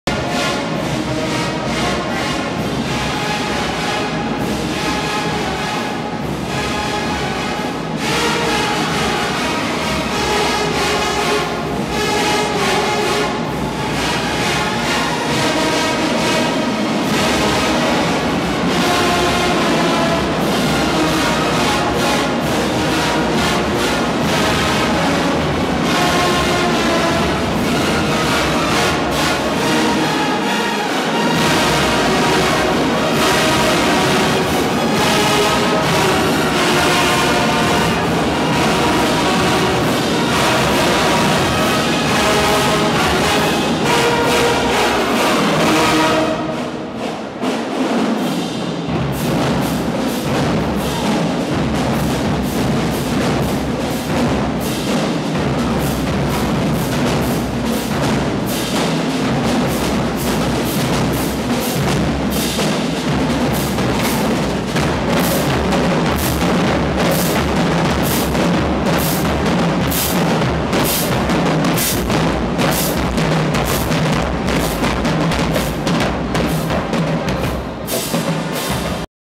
Alcorn State University Marching Band Endymion 2018
Just outside the Super Dome at Mardi Gras, 2018